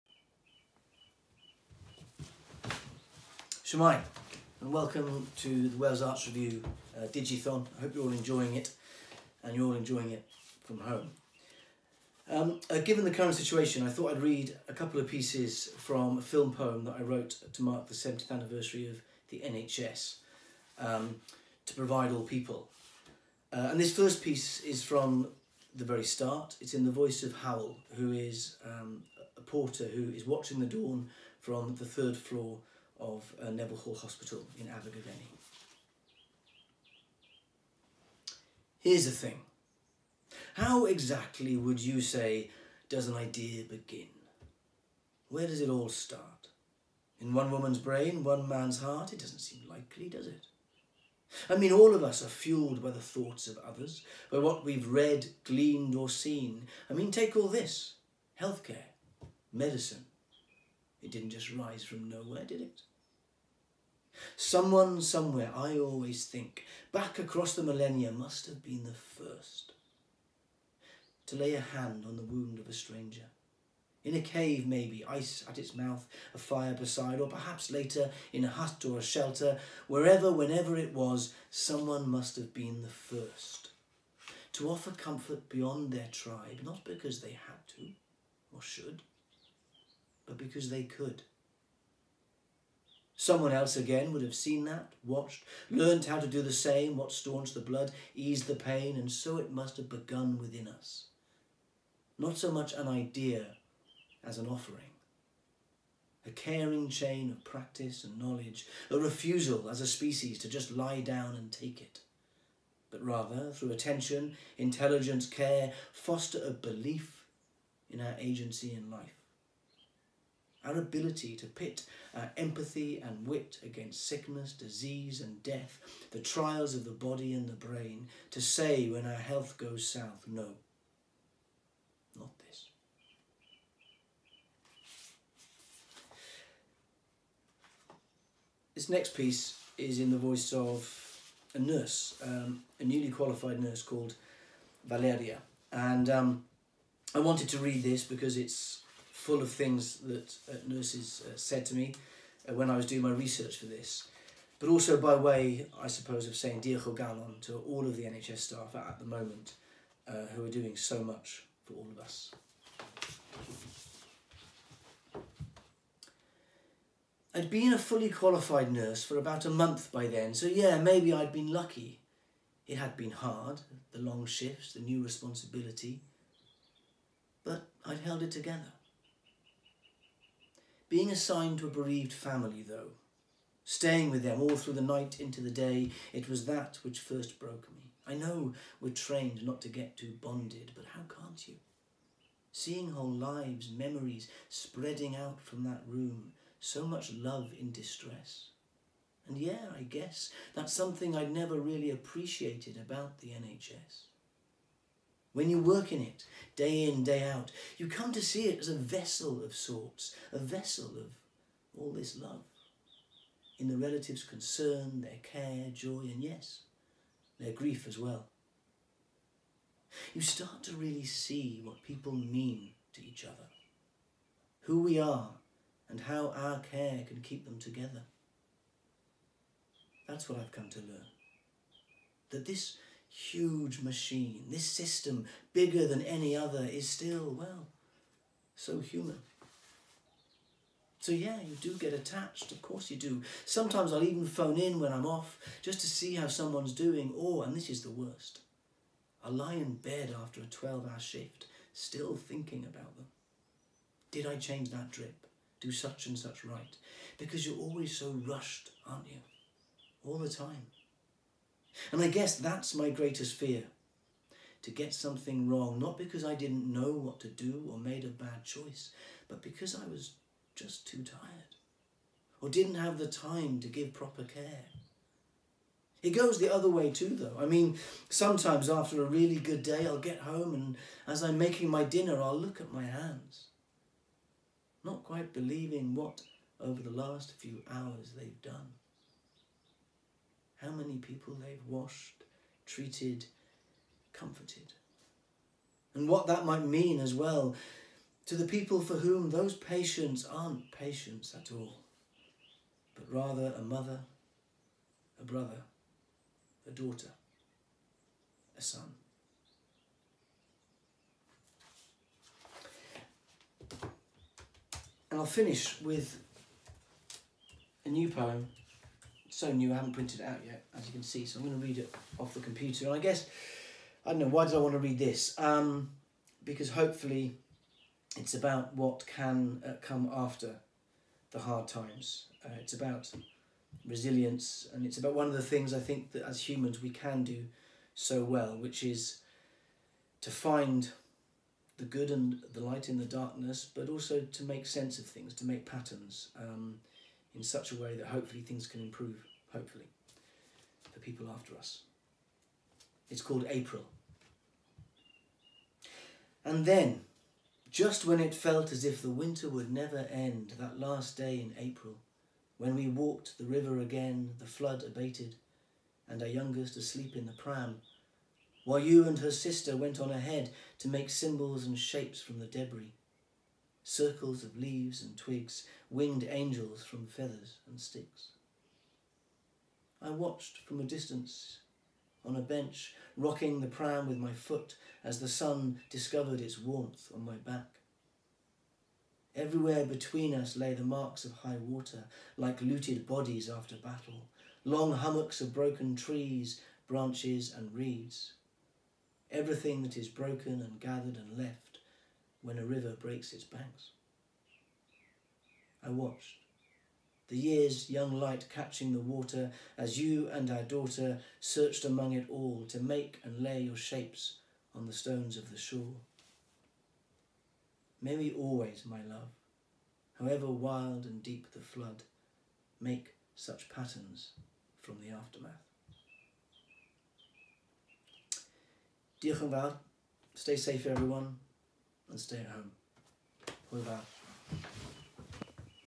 Digithon Lit | Owen Sheers
Owen Sheers reads from his poem for the NHS, To Provide All People.